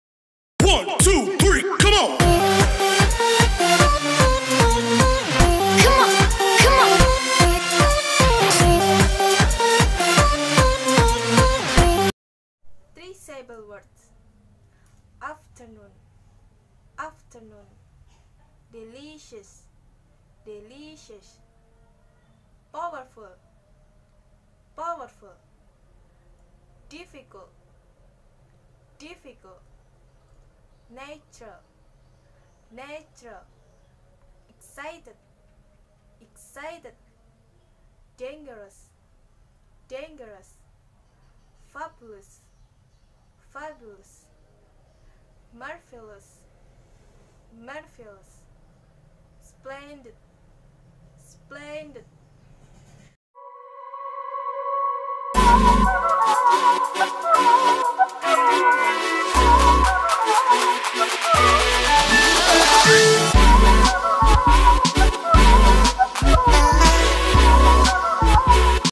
3 SYLLABLE WORD - TUGAS BAHASA INGGRIS #2
Video ini dibuat untuk memenuhi tugas mata kuliah bahasa Inggris saya. Dengan judul 3 sylleble word , semoga bermanfaat bagi yang sudah menonton.
Happy Watching and enjoy😊